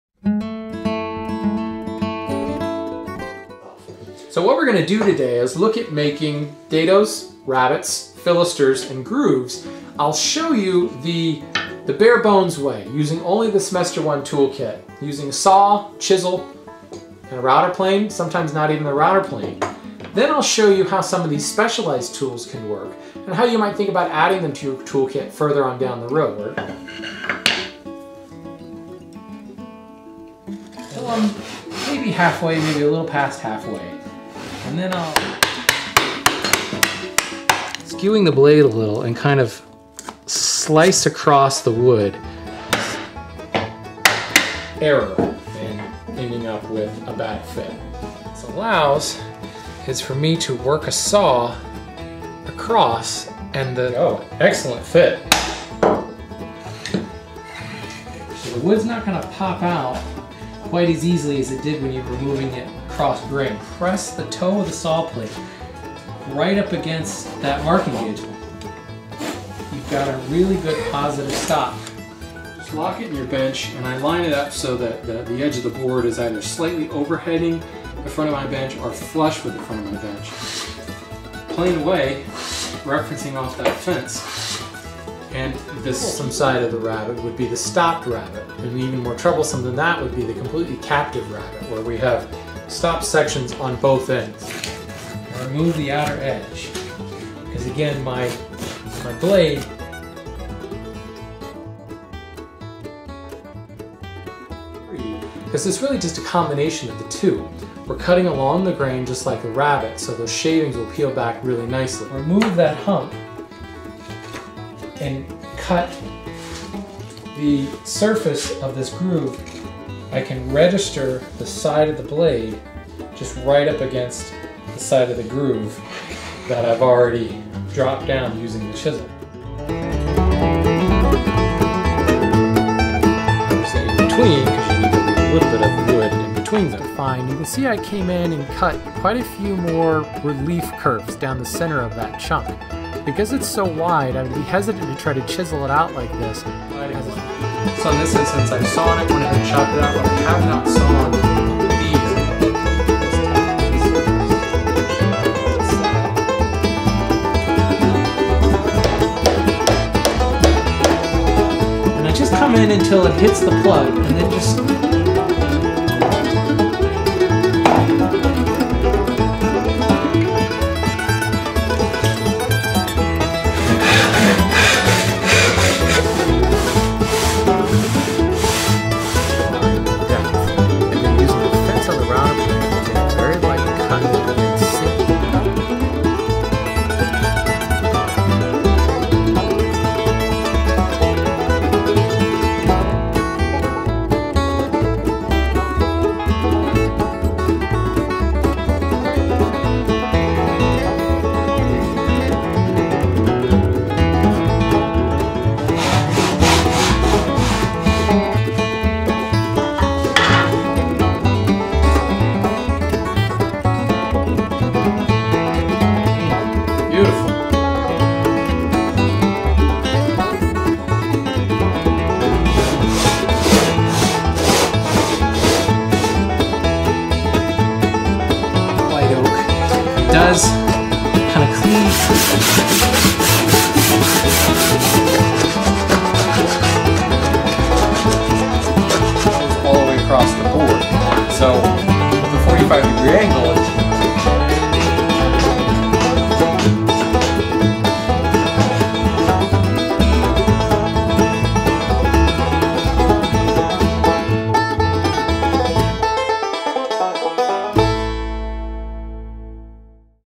Hand Tool School Highlights: Dados, Rabbets, and Grooves
Lesson 4 of The Hand Tool School Semester 1 focuses on dado, rabbet, and groove joinery and demonstrates the most basic way to cut through and blind versions of each joint as well as some techniques using more specialized tools.  We apply those skills by making 3 useful workbench appliances.  Learn more about The Hand Tool School